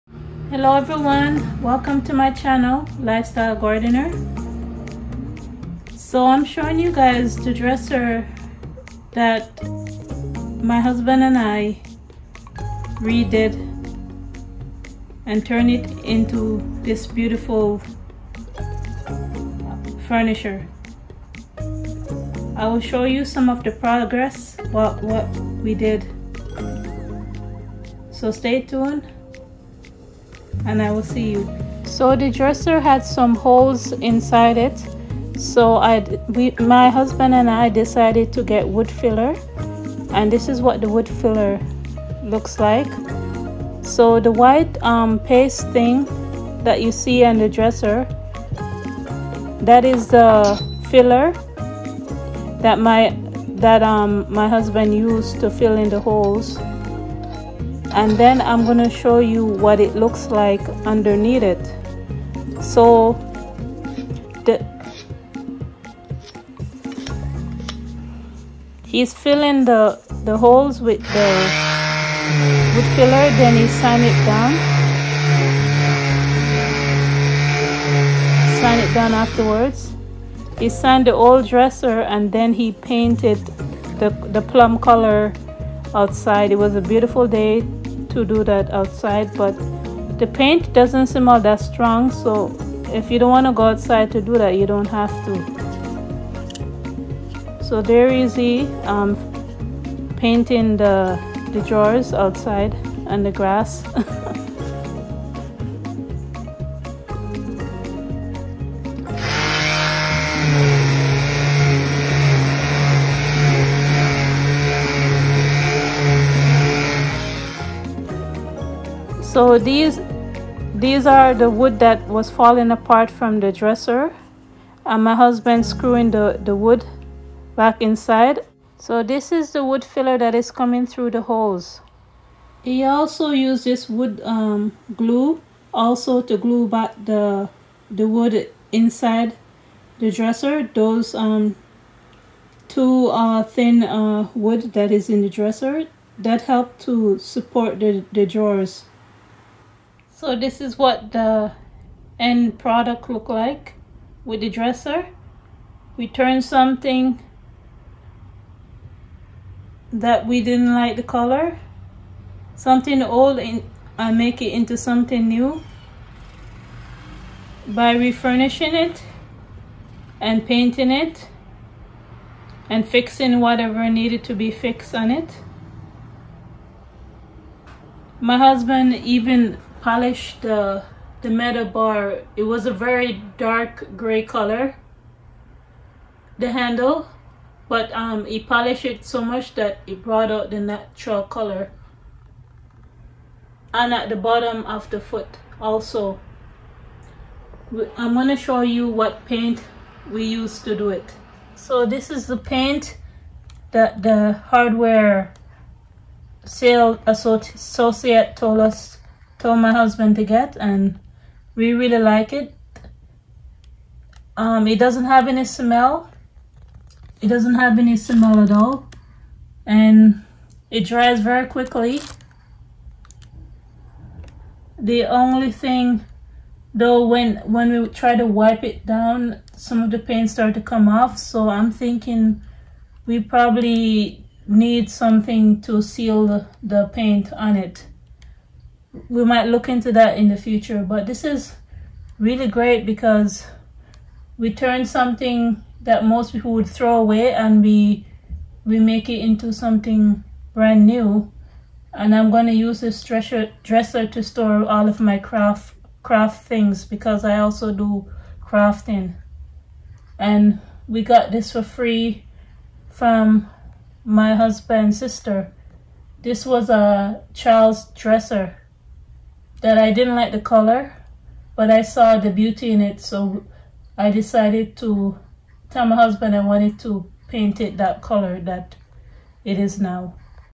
Turning old dresser in to something beautiful!!
Showing how we fix and change the colour of a old dresser!